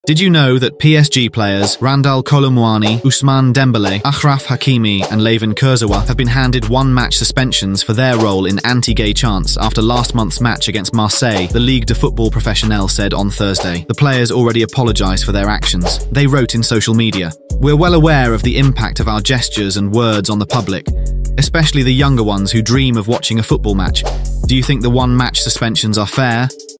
Did you know this psg player are ban from the next game?
This is football

Layvin Kurzawa Kolo Muani Ousmane Dembele and Achraf Hakimi are ban from the next psg game